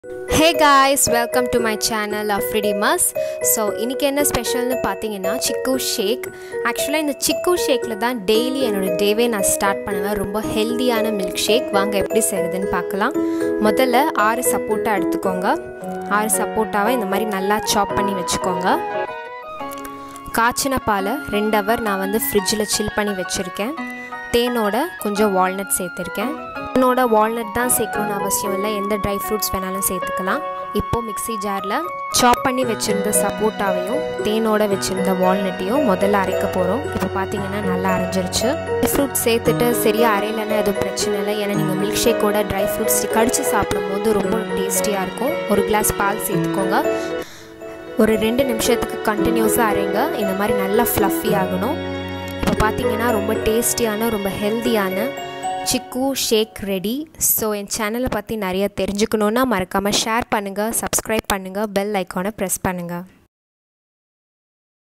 Chikoo shake | healthy breakfast | my day starts with chikoo shake | afride maz
Ingredients:-

Chikoo
Chilled milk
Honey with walnut